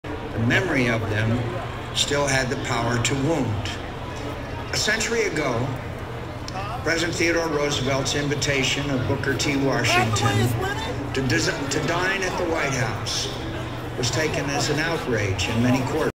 Obama Wins 11/4/08 Grant Park, Chicago
Spontaneous gathering of a million people around 100 giant screen televisions in Grant and Millennium Park in Chicago to watch Obama win the presidential election. This is one group watching McCain's concession speech.